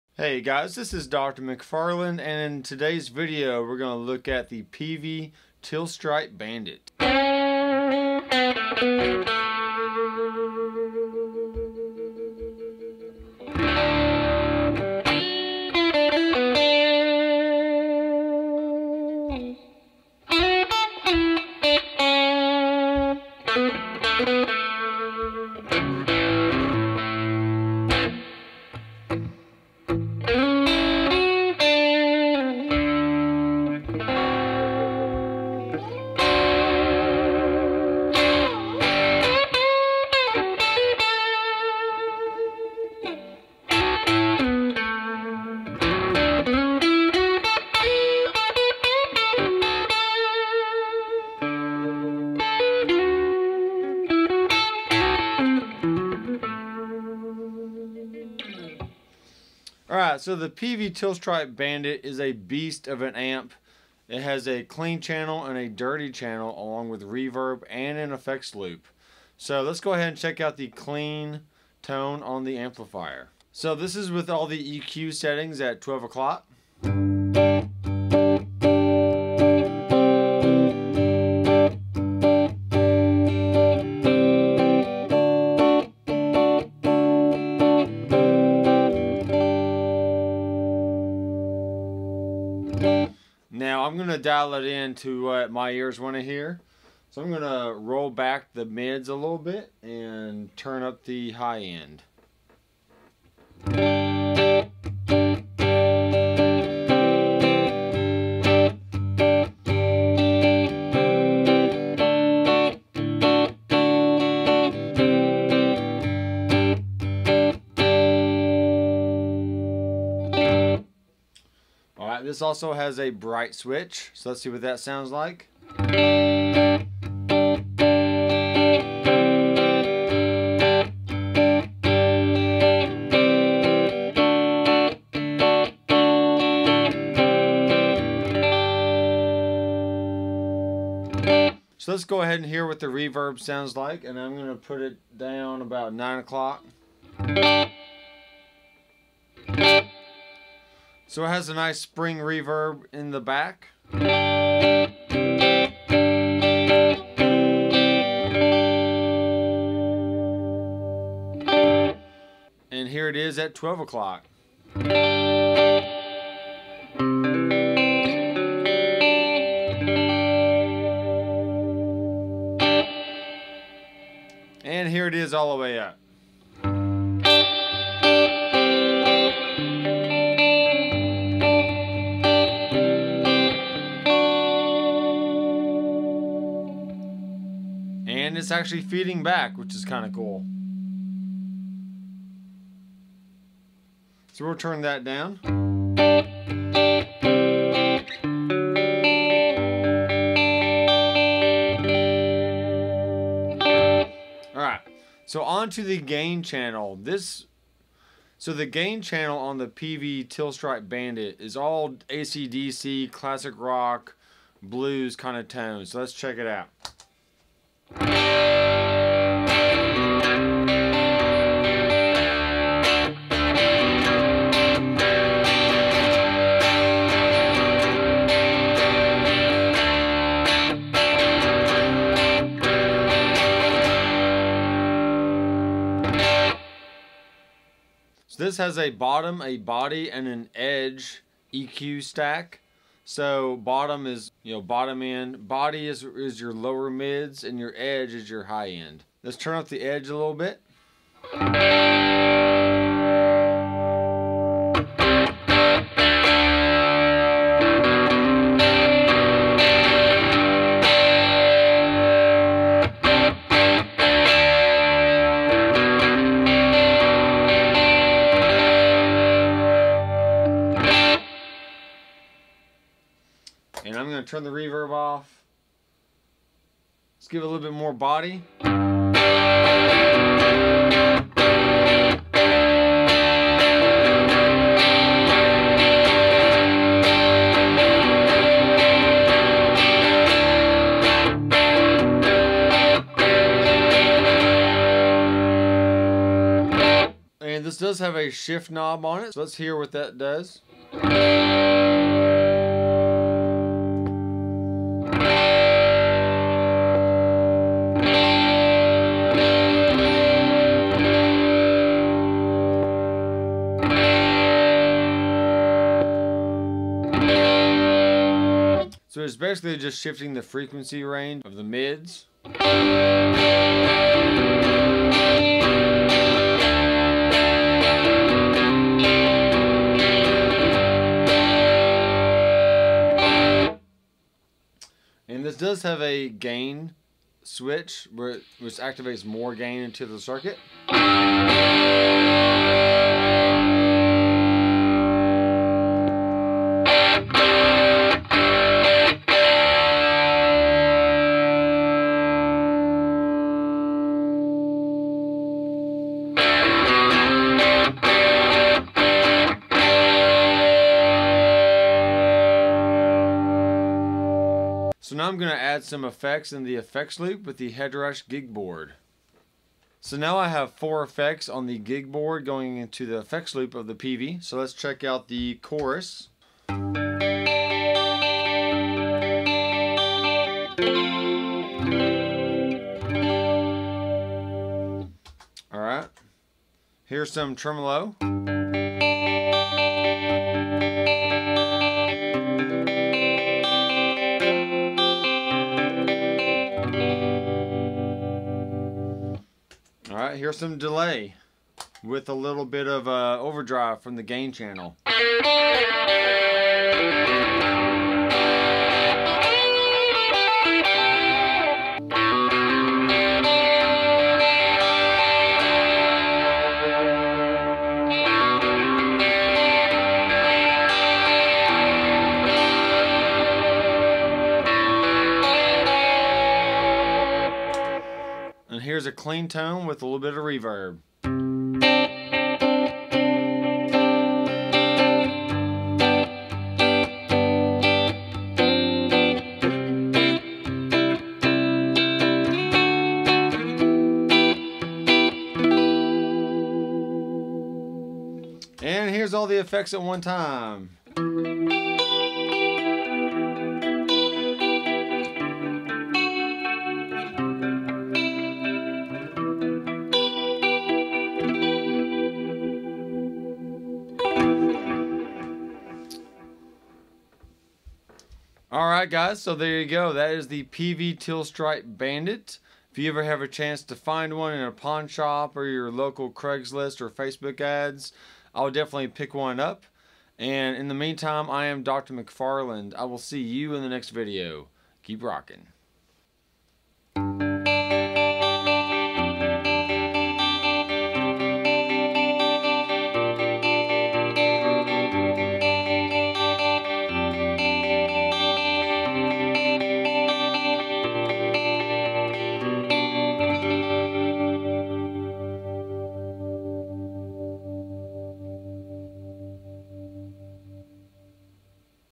Peavey Bandit 112 Teal Stripe Edition
The Peavey Bandit 112 Teal Stripe edition amplifier is a true American made classic.  It has a clean channel and dirty channel with an FX Loop and can be controlled with a footswitch. In this video I will walk you through the setup and tones.  Keep rockin!!!